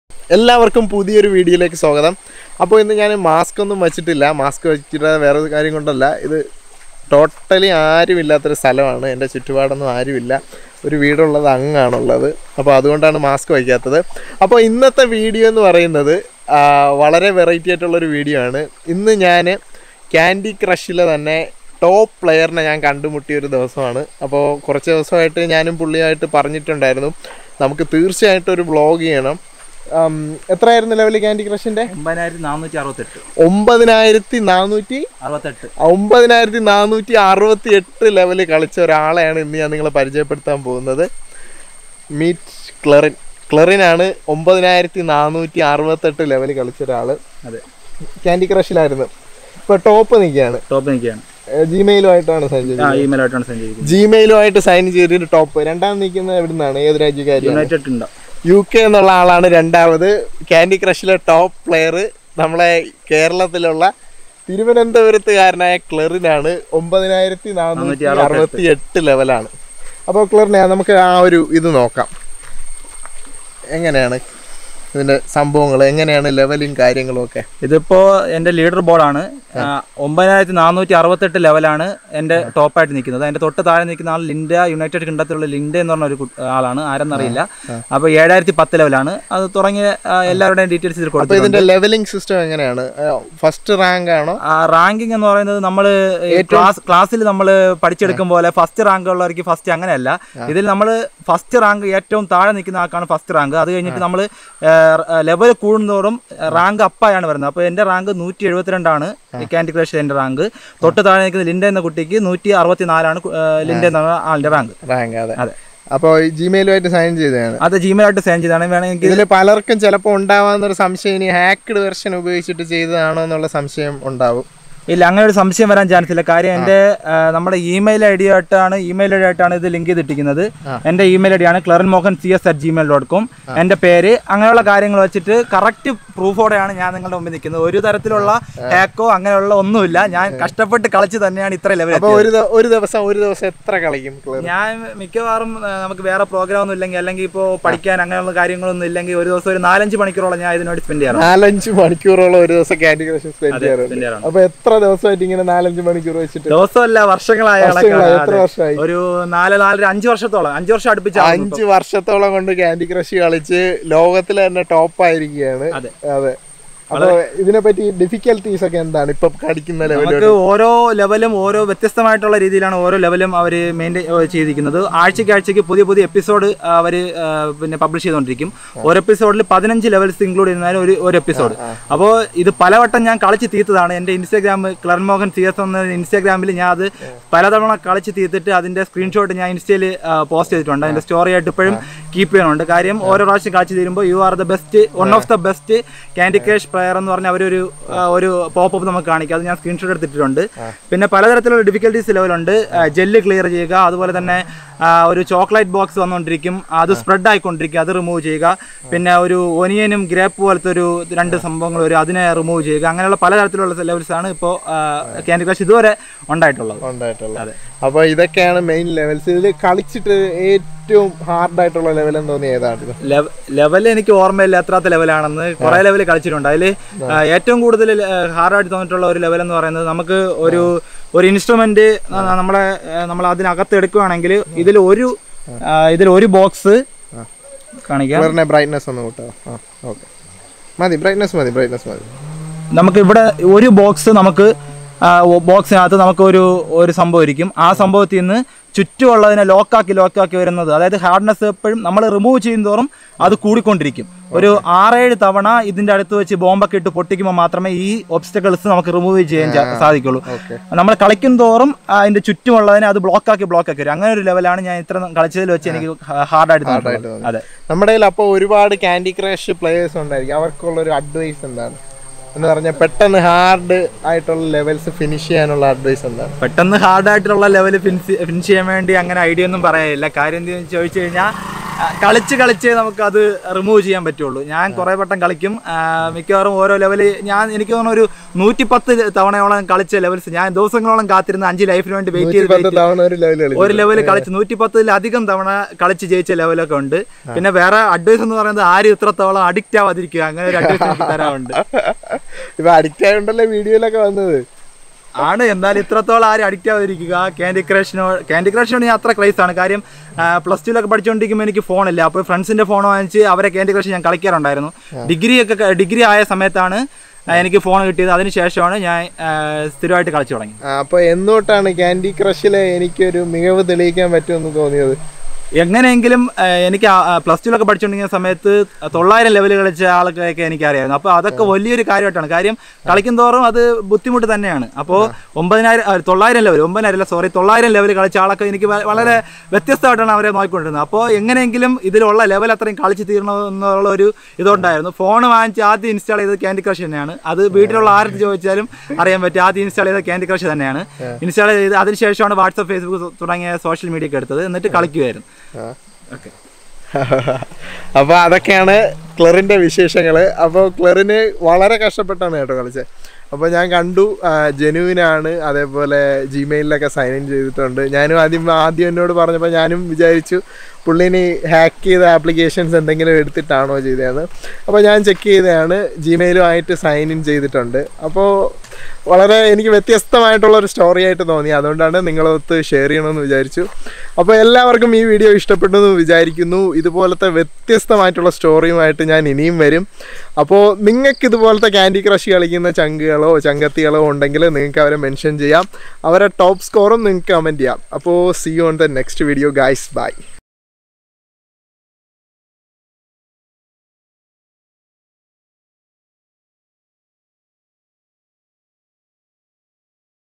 Meet the Candy Crush Highest Level Holder | ടോപ്പ് പ്ലേയർ
Meet Clerin, He is the one holds the highest level player In CANDY CRUSH SAGA as of the upload date. COMMENT YOUR SCORE!